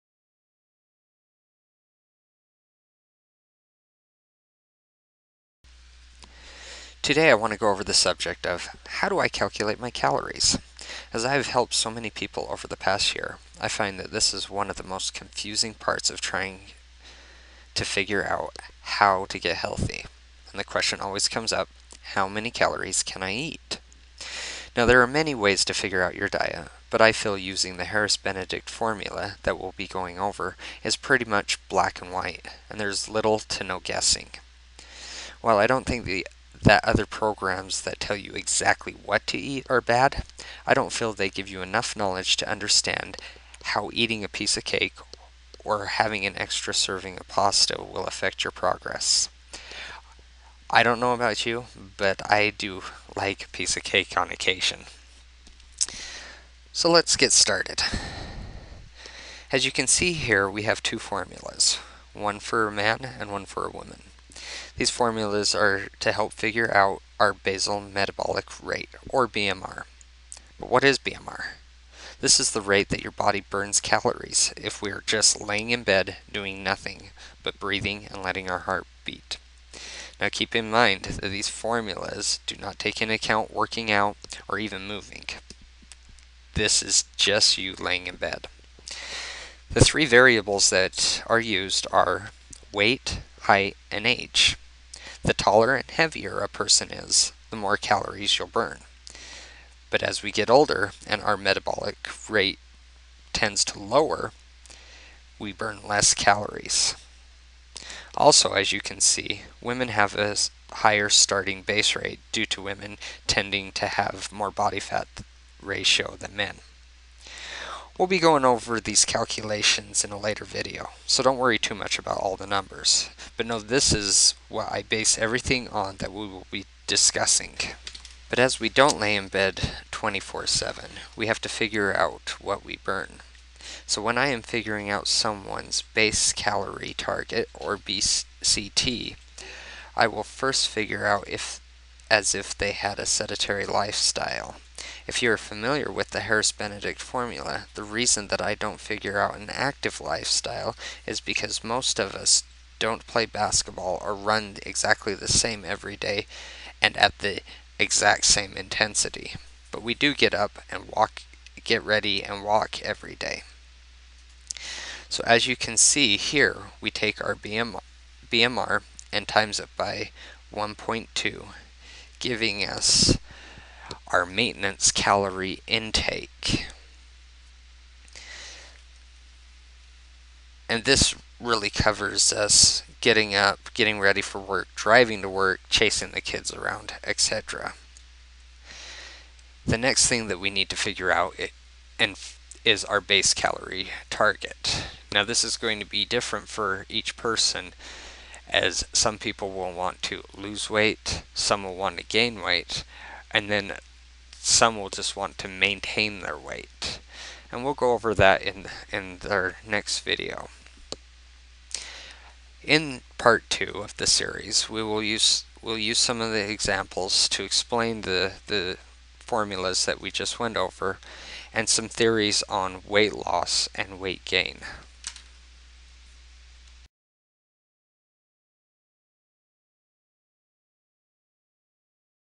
How do I calculate my calories Part 1
Explination on how to calculate your calories when you are try to lose, maintain or gain weight.